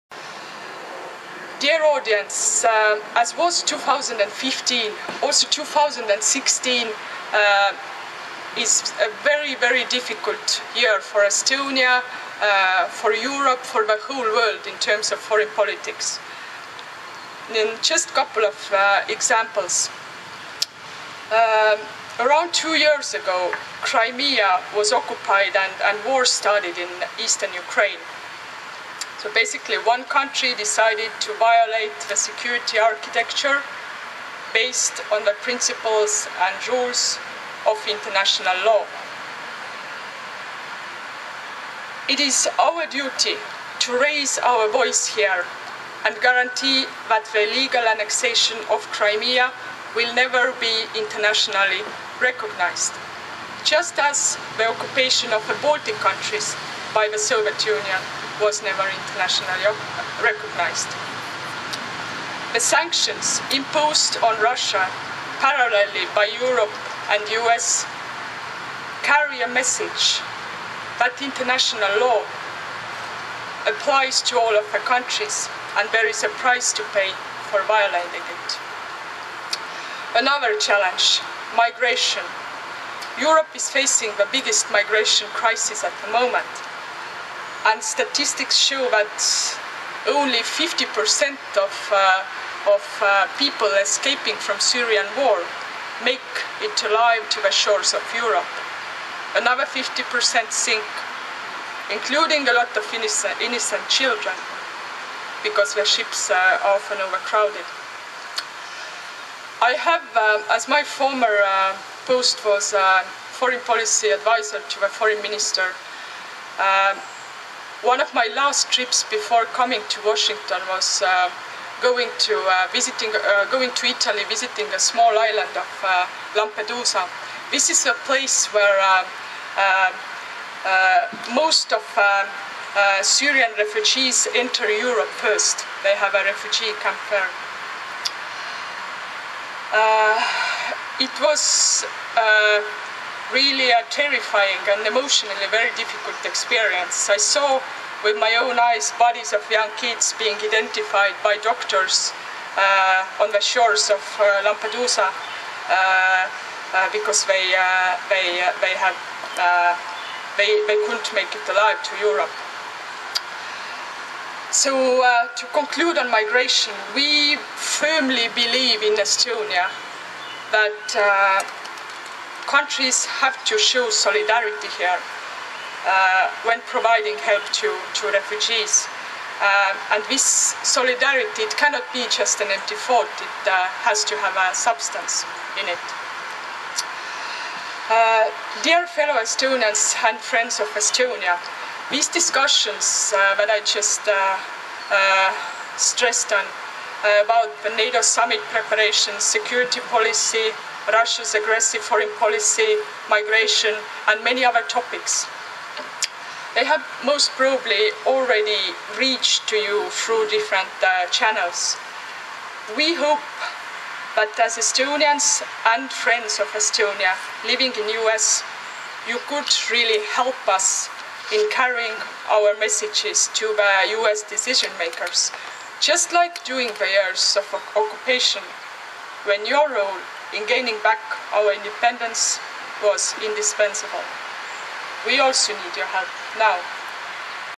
Estonian Embassy Chief in Cleveland – Crimea, Ukraine, Migration
The Honorable Mrs. Marki Tehhonova-Kreek, Deputy Chief of Mission of the Estonian Embassy, Washington DC as keynote speaker at the 50th anniversary celebration of the Estonian Cultural Garden in Cleveland Ohio.  It was also the annual celebration of Victory Day (Võidupüha) and St. John’s Day (Jaanipäev). This part of her speech was about Crimea, Ukraine and the migration crisis.